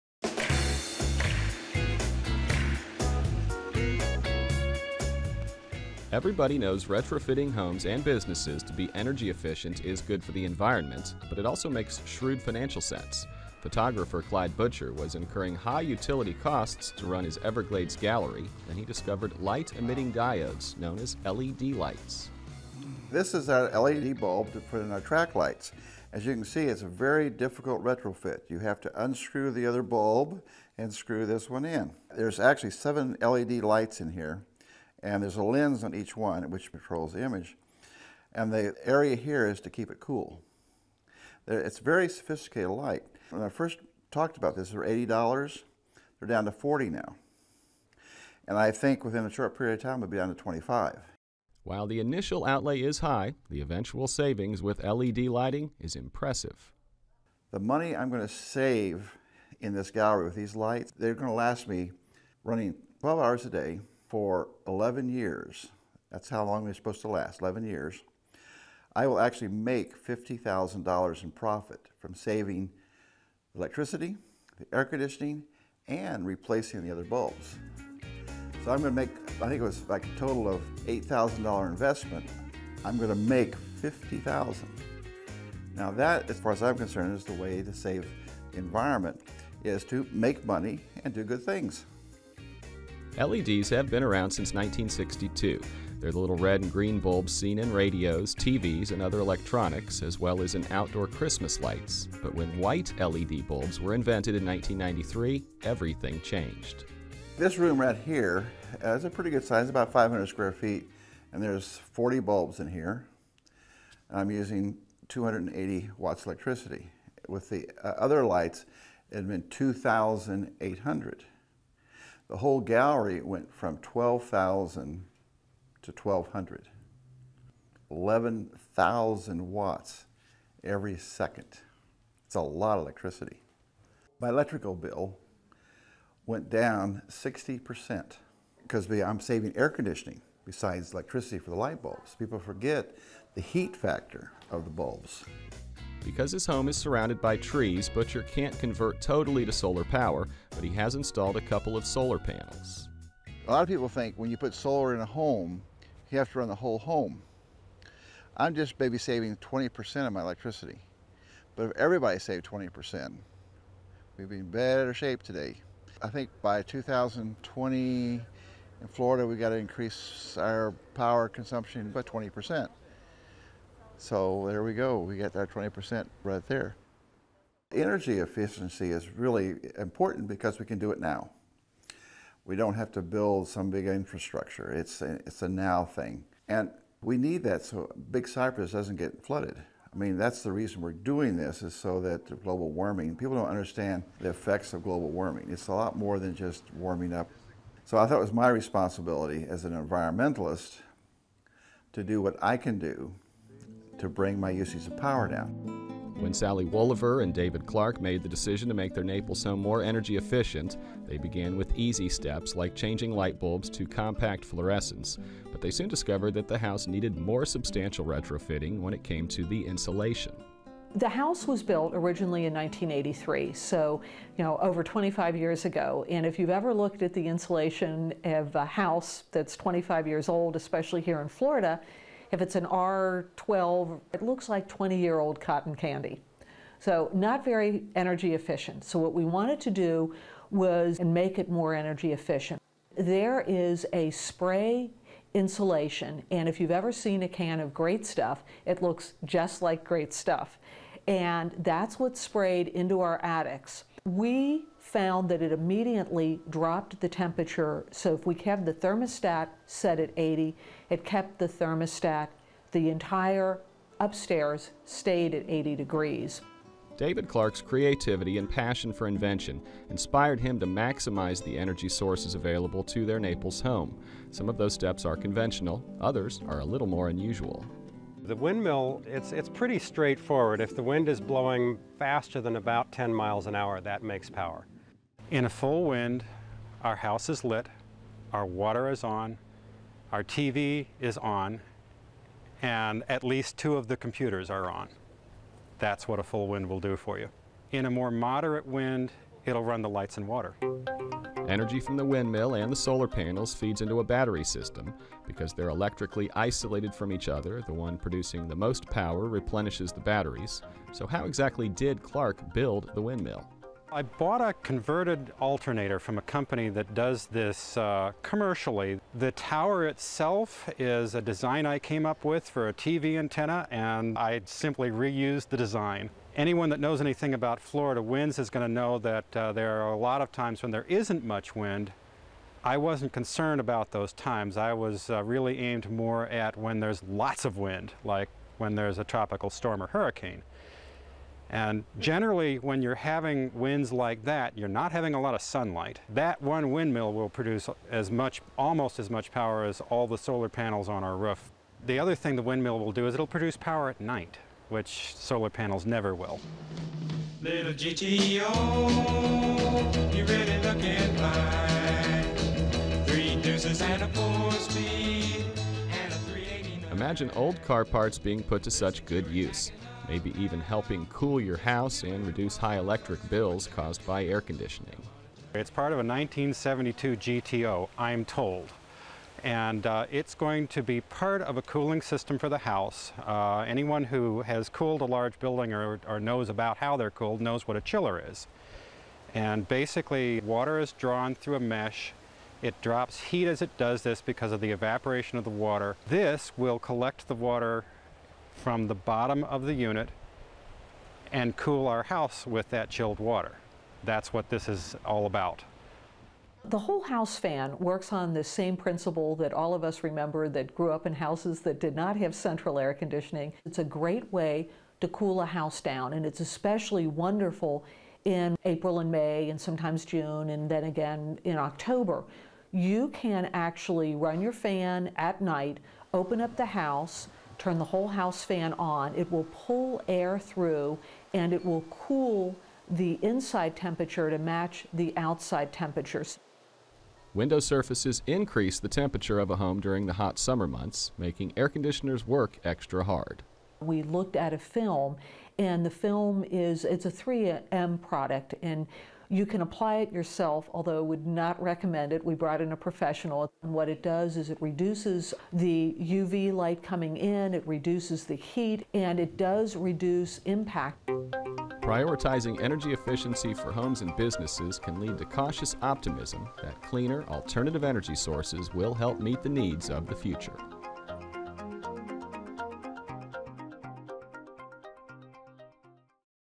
Retrofit to Green
Writer/Producer: Rosie Emery

Retrofitting homes and business to be more sustainable makes financial sense and helps the planet.  Renowned photographer Clyde Butcher was amazed at the savings he incurred when he installed the highly efficient LED light bulbs into his gallery.  Naples residents, Sally Woliver and David Clark share some of the innovative ways in which they have transformed their home to maximize alternative energy sources.

Flex Your Power website  tips on how to save energy and retrofit your home

The insulation that Sally Woliver refers to is Icynene - a revolutionary soft, breathable foam insulation that is proven to better reduce air-borne moisture movement and entrapment. Heating and cooling costs can be reduced by 30-50%.